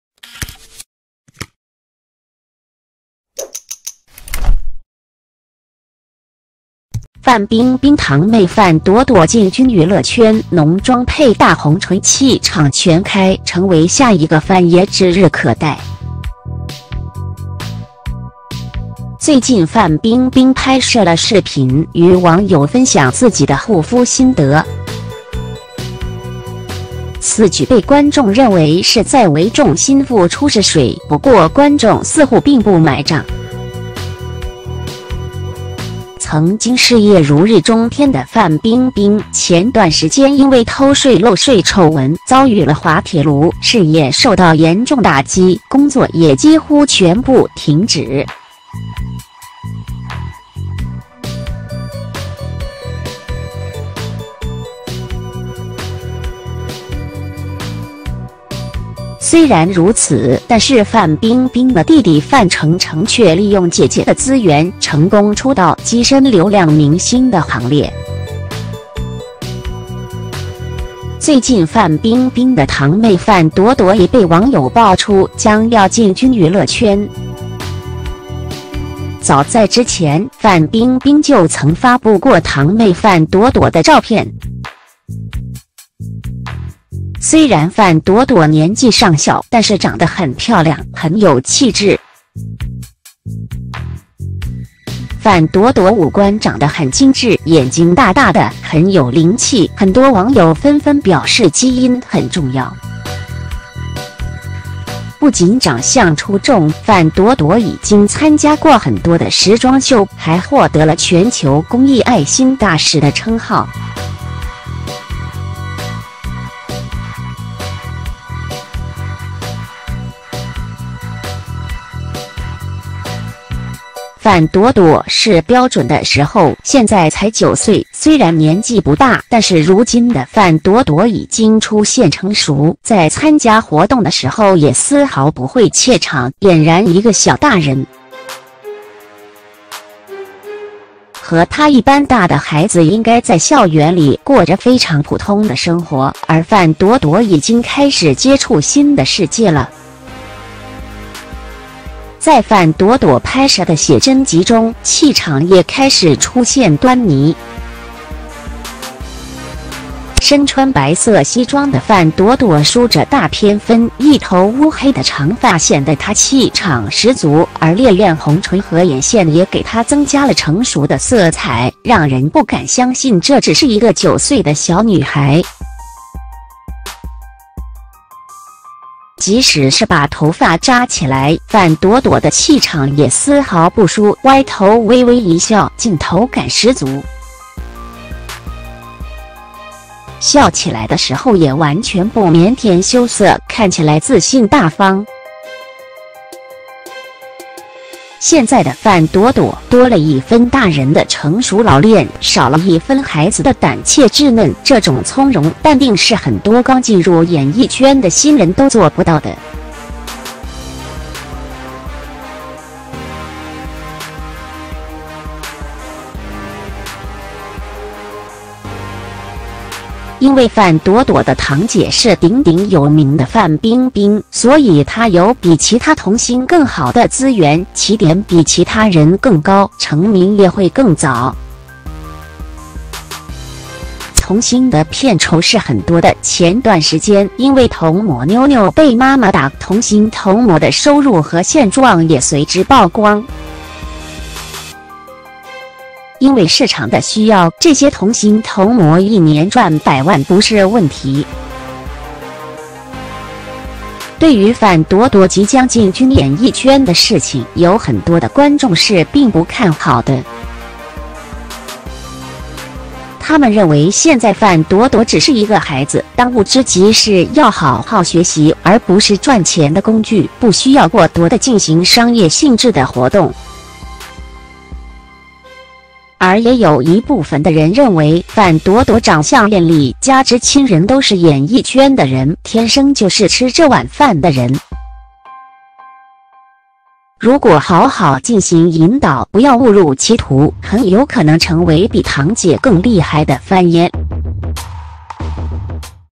范冰冰堂妹范朵朵进军娱乐圈，浓妆配大红唇气场全开，,成为下一个范爷指日可待
范冰冰 专访 管理好情绪#“明星效应”#“黑粉抨击”